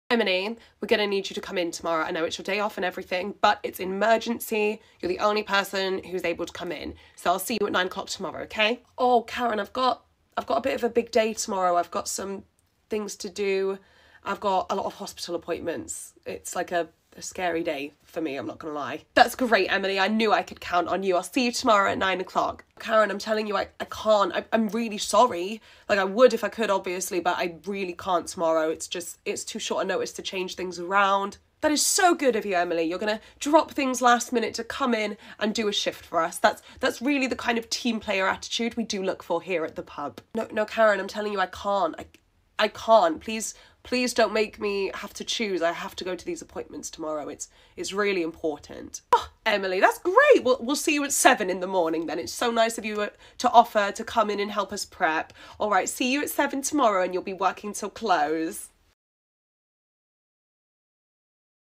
The manager trying to take my "NO" to mean "YES"| AC
I've tried explaining to the manager that I can't make it to work on my day off on such a short notice. But she chooses to guilt trip me using the "team" tag so that I come to work.